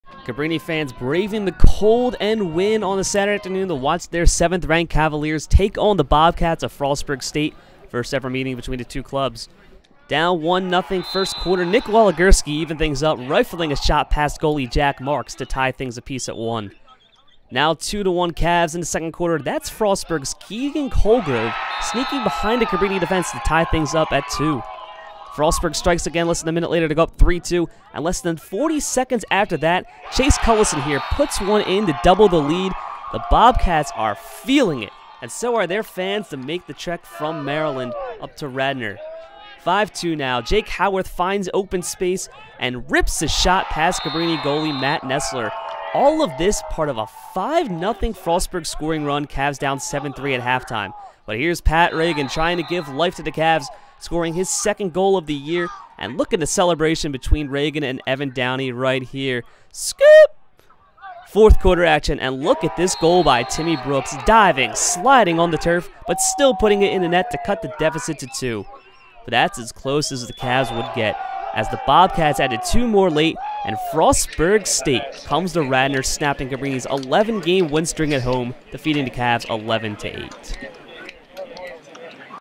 Men's Lacrosse Takes On Frostburg State
The No. 7 Cabrini Cavaliers hosted Frostburg State for the first time in program history, hoping to avoid a two-game losing streak.

Video by Brian Nestel.